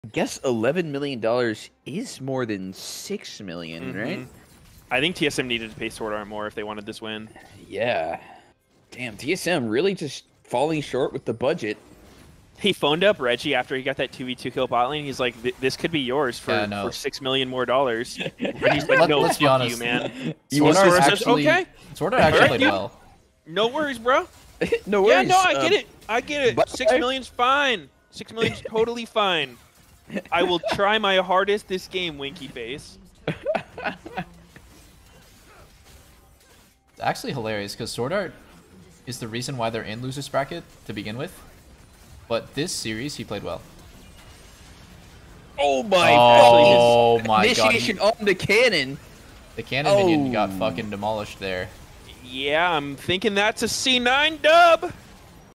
Doublelift, Sneaky, and Meteos Reveal the Real Reason TSM Lost
Severe budget gap. Doublelift funny Co Stream clip with Sneaky and Meteos. TSM vs C9.